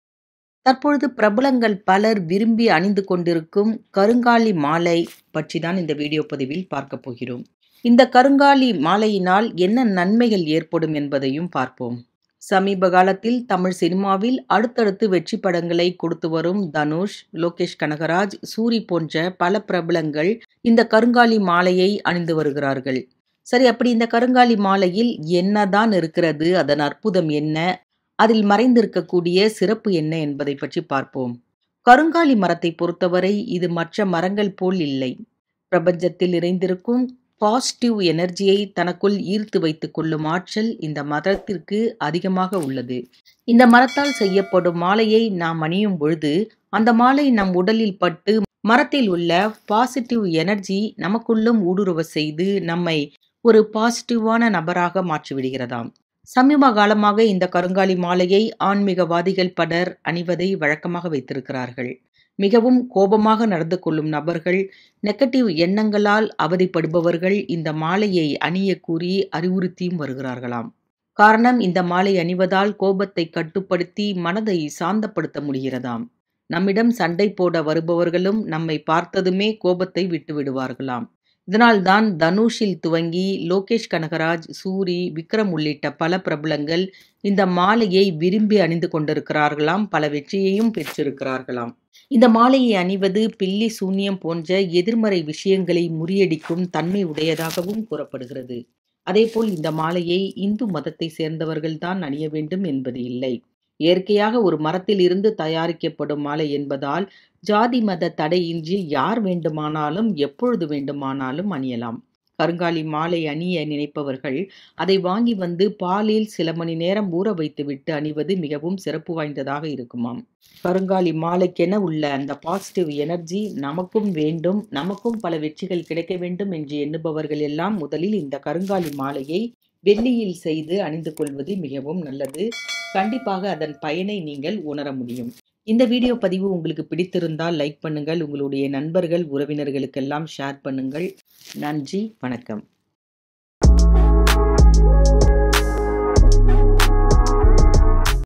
கருங்காலி மாலை..! திரை பிரபலங்கள் விரும்பி அணிய என்ன காரணம்..? / Karungali Malai Benefits In Tamil
கருங்காலி மாலை..! திரை பிரபலங்கள் விரும்பி அணிய என்ன காரணம்..? /  Karungali Malai Benefits In Tamil / Dhanush / Lokesh Kanagaraj